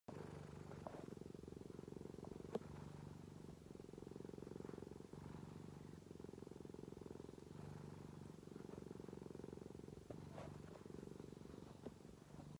Badger - Worthing Cat Welfare Trust
Badger is available for adoption.